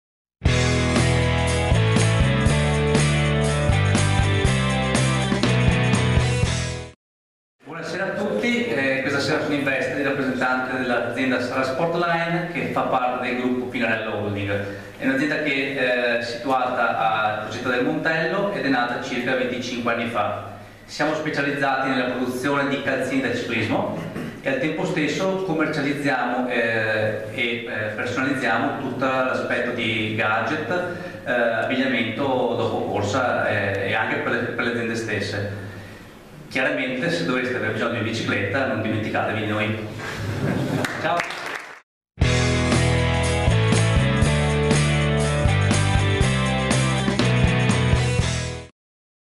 TIME OUT - Nicola Dalla Pietà Pinarello presenta "Sara Sport Line"
Al via il contest “Time Out”. I consorziati di UniVerso Treviso hanno avuto 1 minuto di tempo per presentare la loro realtà. Ora tocca a te scegliere quella più interessante, fantasiosa, simpatica, insomma, quella che ti piace di più! Hai tempo fino al 9 marzo per votare il tuo video preferito sulla pagina facebook Consorzio UniVerso Treviso. Il vincitore sarà premiato al Palaverde prima di una partita di Treviso Basket e ospiterà in azienda una delegazione della squadra.